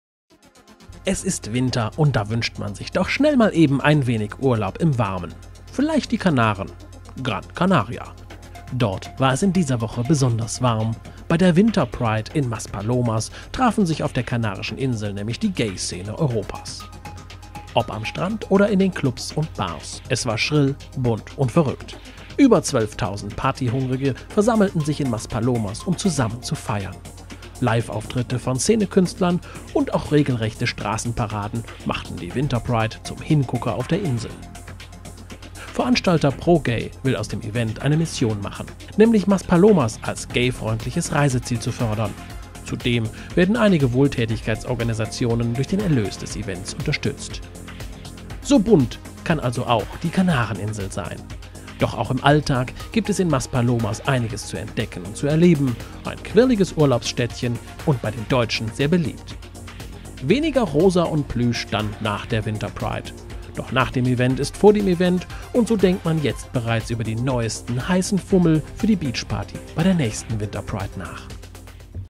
Winterpride in Maspalomas - Fast so wie Karneval?
Immer im November startet die große Winterprime auf Gran Canaria in Maspalomas. Eines der spektakulärsten Gay Events in Europa. SO sieht es dann in Maspalomas aus...Fast wie Karneval in Deutschland...